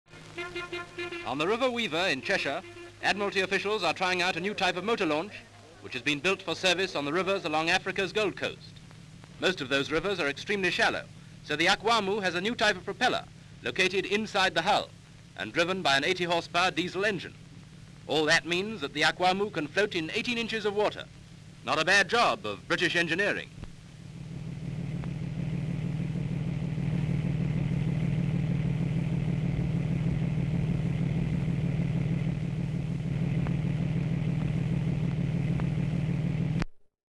Trying Out New Type Motor Launch On River Weaver (1938)
Northwich, Cheshire.

Several shots of a new kind of motor launch being tried out on the River Weaver on a rather foggy day.  The ship is called the 'Akwamu' has been built for service on the rivers of Africa's Gold Coast
 FILM ID:949.36